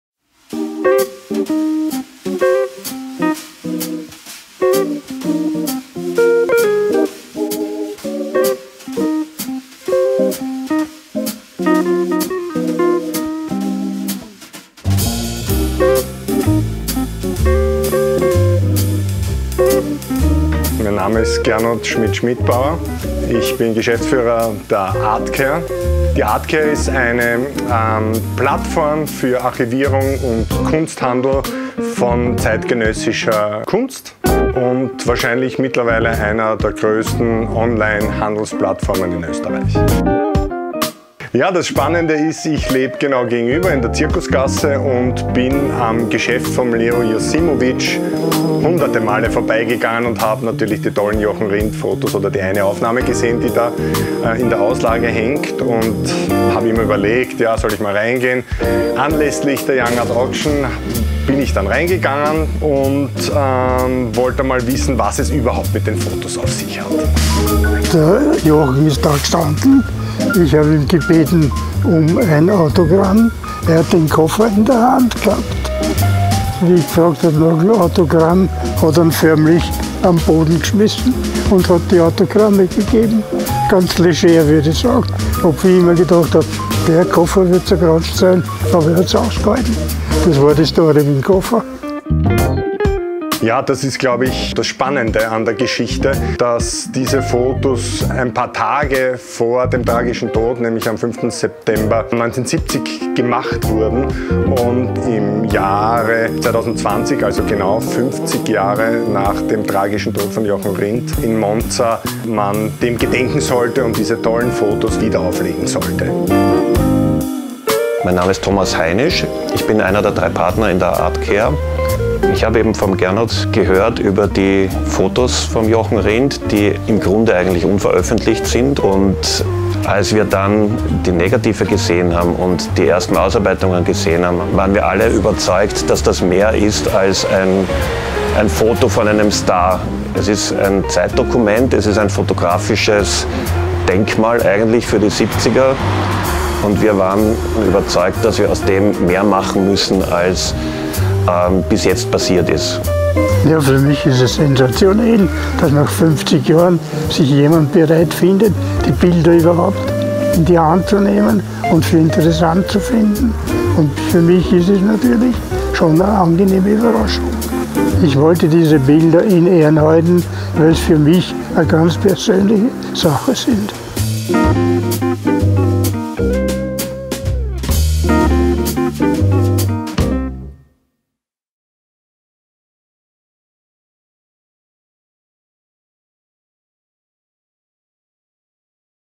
Jochen Rindt last private photos by leo josimovic powered by artcare.at
Jochen Rindt turned up in Leo Josimovic`s car radioshop one day. "My car radio is going crazy," he said. Josimovic looked after the radio, meanwhile Rindt had a suit measured for him across the street at Licona`s. The damage was quickly repaired, only one plug was loose. At the end Leo took photos with his Hasselblad, which led to a crowd of people in the street. That was on a Thursday, Josimovic recalls. On Saturday, two days later, Rindt crashed at the Monza Formula 1 Grand Prix circuit.

Get one of Jochen Rindts last photos. Leo Josimovic who took some of the last private photos of the F1 World Champion.